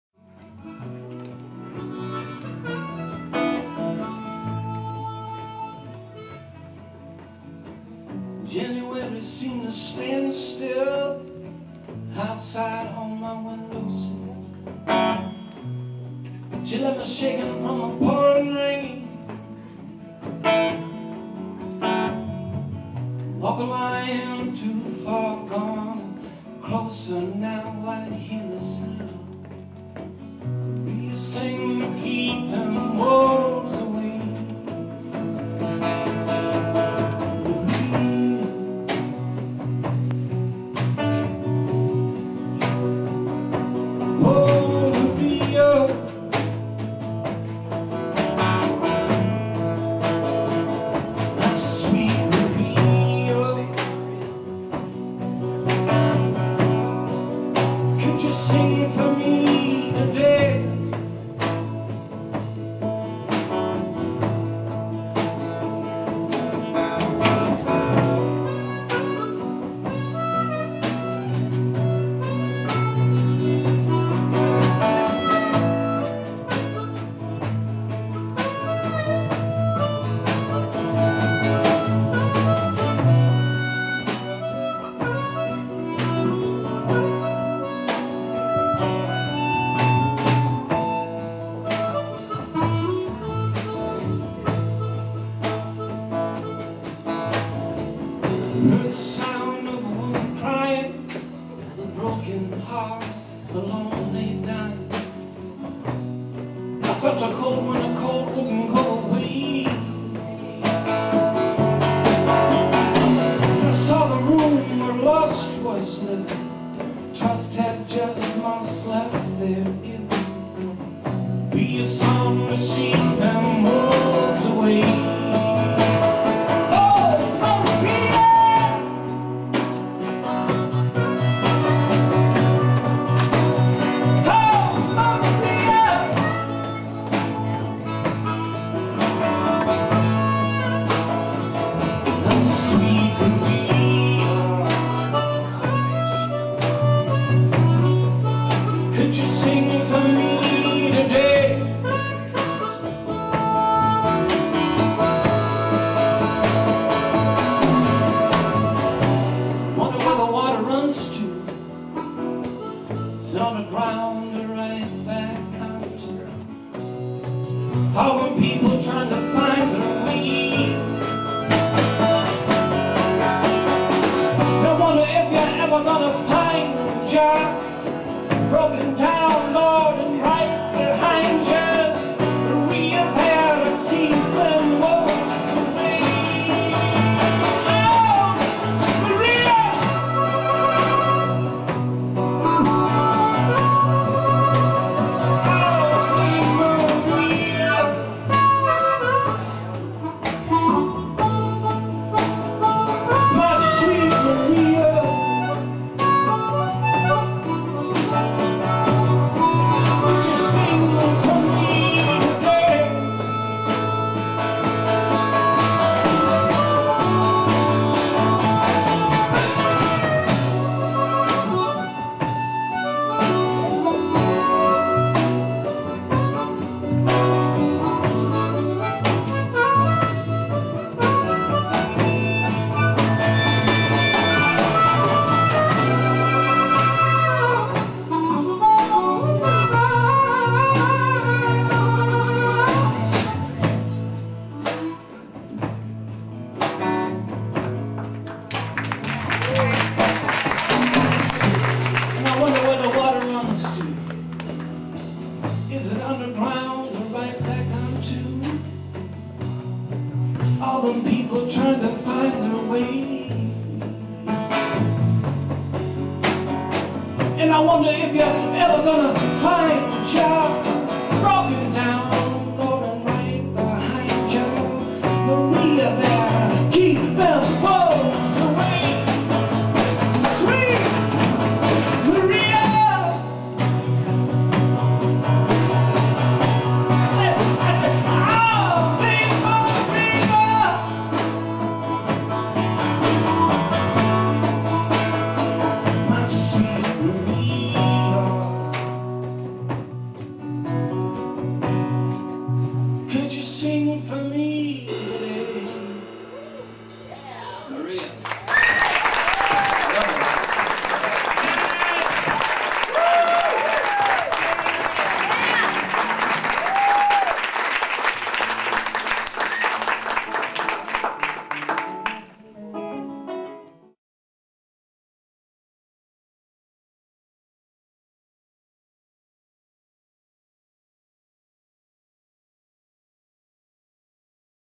Sugar & Spice - Dusty Pascal
Singer, songwriter Dusty Pascal performs at WAER's Sugar & Spice holdiay concert.